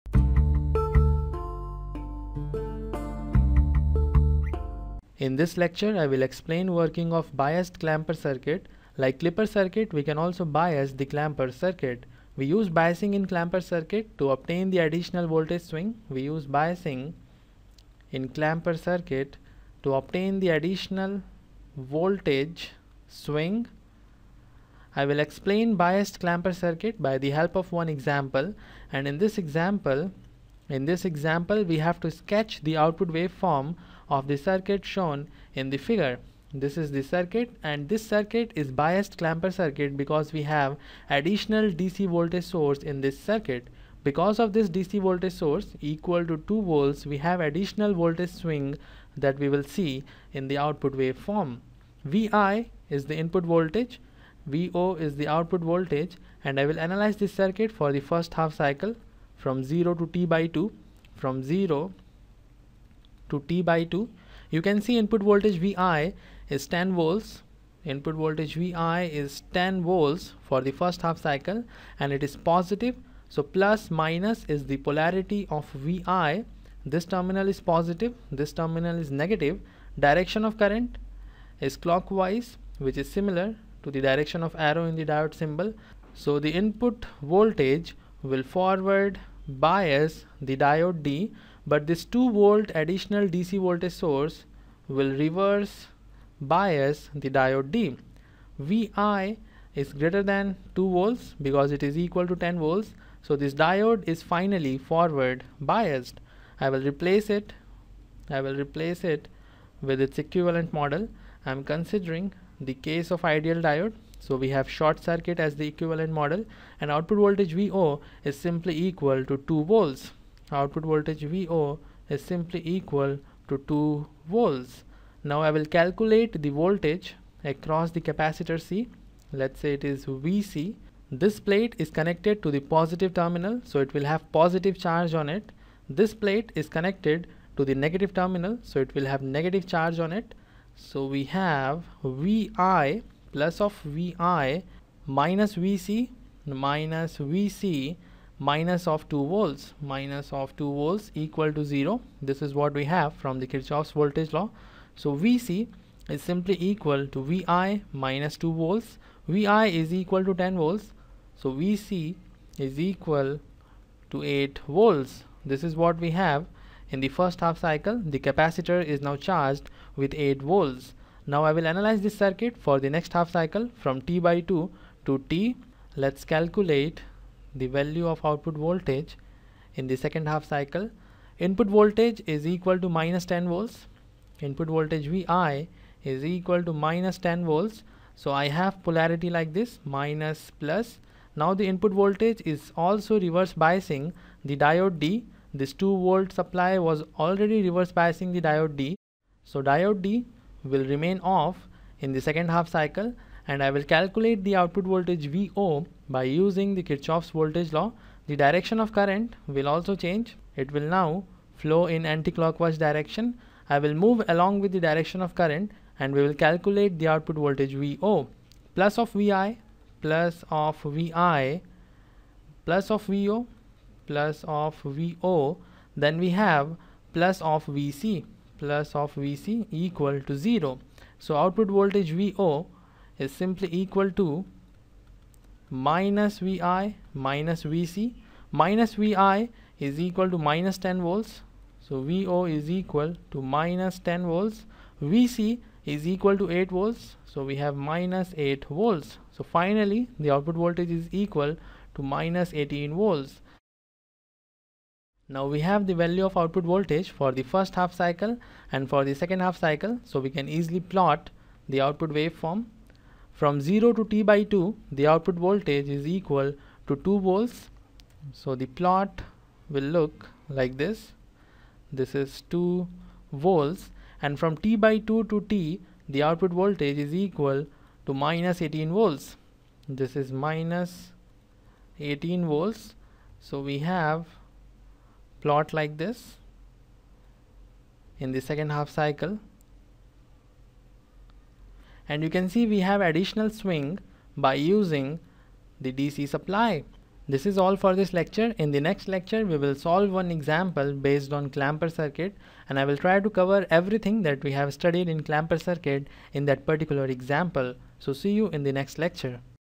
Biased Clamper Circuit
Analog Electronics: Biased Clamper Circuit
Topics discussed:
1. Biasing in clamper circuit.
2. Solved example of biased clamper circuit.